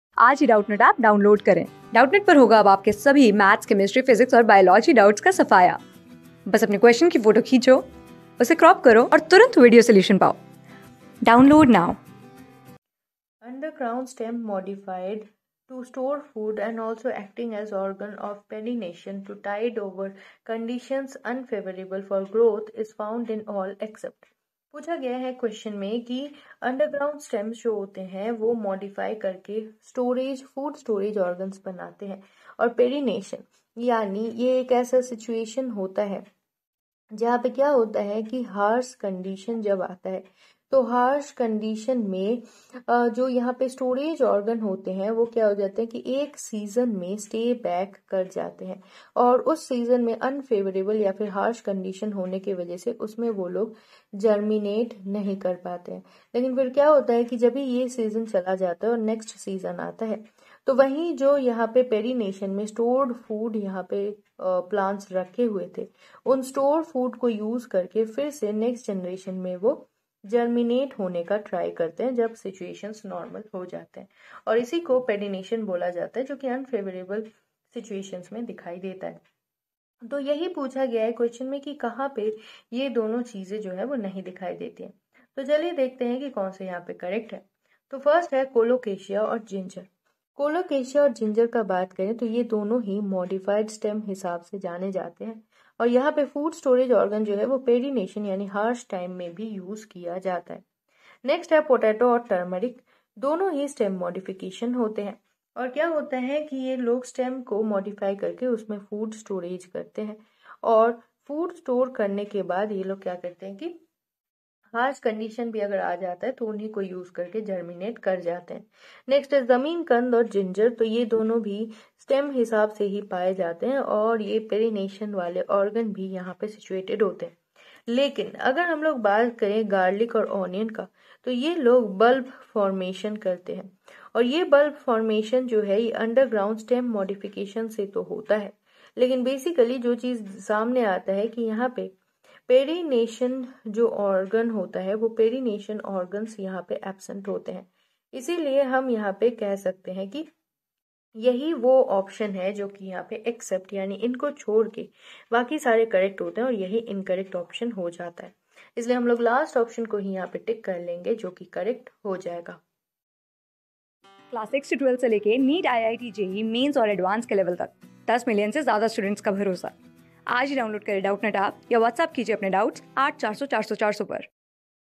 Underground stem modified to store food and also acting as organs of perennation to tide over co...
Underground stem modified to store food and also acting as organs of perennation to tide over conditions unfavourable for growth is found is all except
Class: 12
Subject: BIOLOGY
Chapter: TEST 4
Board:IIT JEE

👉 Have Any Query? Ask Us.